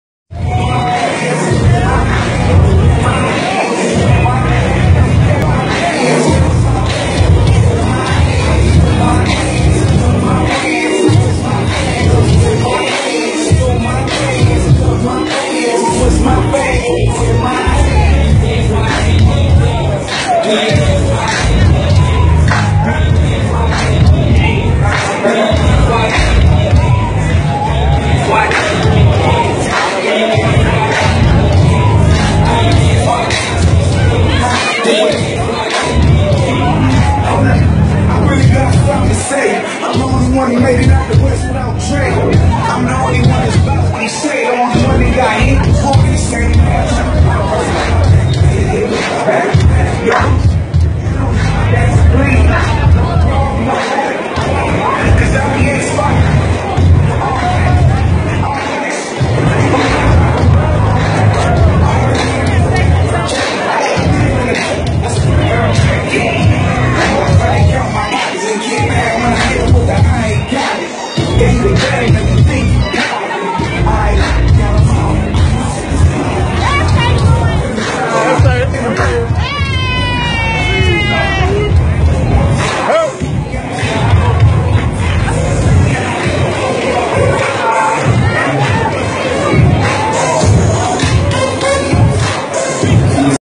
How To Control The Rink STL Style Gliding
Blessing Texas with smooth rollers from the Lou STL (St Louis) Gliding controlling the rink!